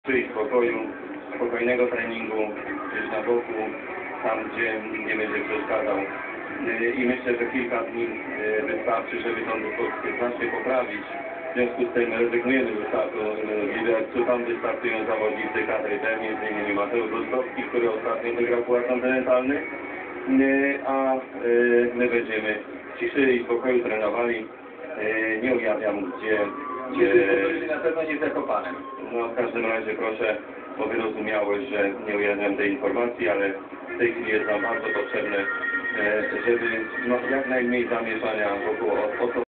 A.Tajner ogłasza skład na Liberec 03/04
Skoki narciarskie. Sezon 2003/04. W materiale tym Aploniusz Tajner ogłasza skład na konkursy w Libercu.